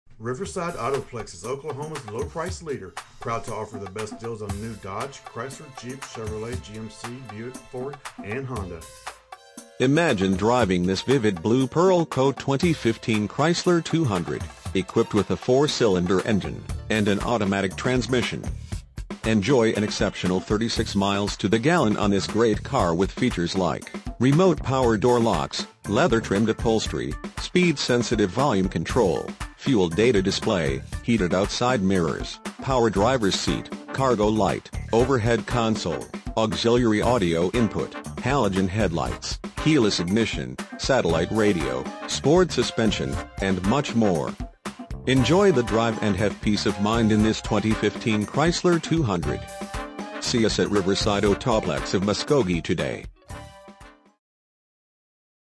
2015 Chrysler 200 Riverside Autoplex of Muskogee Muskogee, OK 74401
Chrysler Imagine driving this Vivid Blue Pearl Coat Chrysler 200 4 Dr Sedan, equipped with a 4 Cylinder engine and an automatic transmission. Enjoy an exceptional 36 miles to the gallon on this great car with features like Sirius Xm Satellite Radio, Halogen Headlights, Auxiliary Audio Input - Ipod/Iphone Integration, Push-Button Start Keyless Ignition, Ambient Lighting, Electronic Stability Control, Heated Exterior Mirrors, Anti-Theft Alarm System With Engine Immobilizer, Remote Anti-Theft Alarm System, Leather-Trimmed Upholstery, 4-Way Power Adjustable Lumbar Driver Seat, Electronic Brakeforce Distribution, Front Fog Lights, Speed Sensitive Central Locking Power Door Locks, Emergency Braking Assist, Premium Cloth Upholstery, Electric Power Steering, 8 Way Power Driver Seat, and much more. Enjoy the drive and have peace of mind in this Chrysler 200. See us at Riverside Autoplex of Muskogee today!
MILES:     12
VIN:1C3CCCBB4FN531318

1711 West Shawnee
Muskogee, Oklahoma 74401